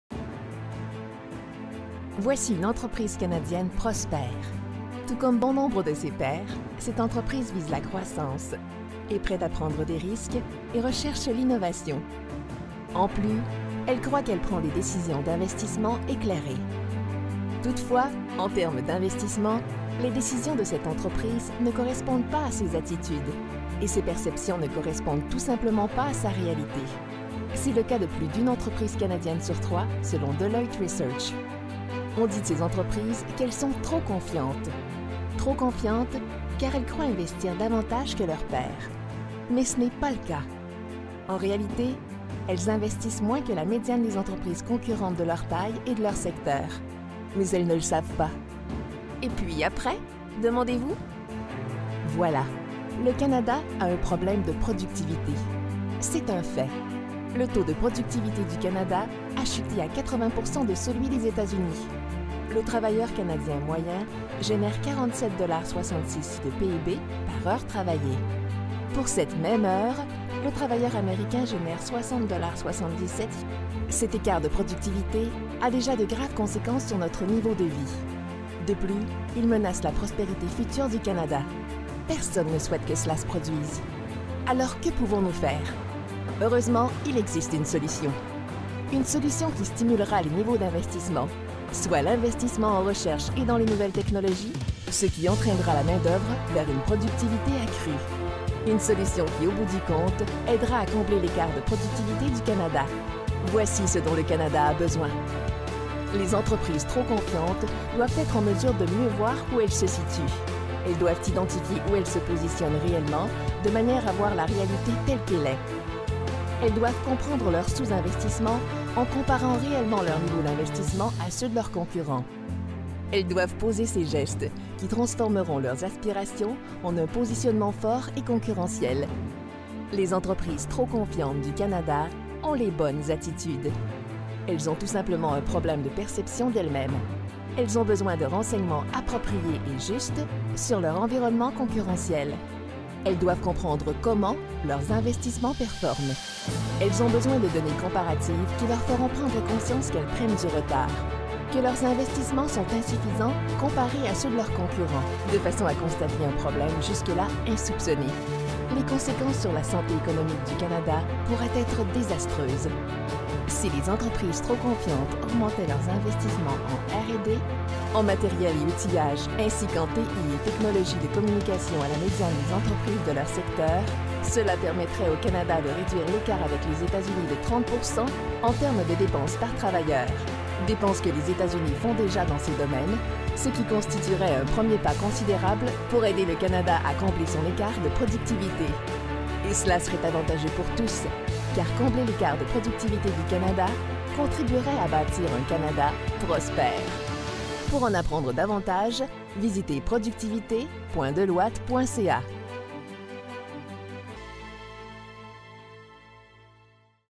L'avenir de la productivité 2013 : Comblez l'écart de perception
En quoi consiste l'écart de productivité du Canada? Le taux de productivité du Canada est-il aussi bas qu'on le prétend? Votre entreprise investit-elle suffisamment dans sa productivité? Cet écart de productivité a déjà de graves conséquences sur notre niveau de vie. De plus, il menace la prospérité future du Canada. Pourtant, un bon nombre d'entreprises possèdent les bonnes attitudes et investissent dans leur productivité. Alors comment expliquer l'écart de productivité du Canada? Pour le savoir, visionnez la vidéo.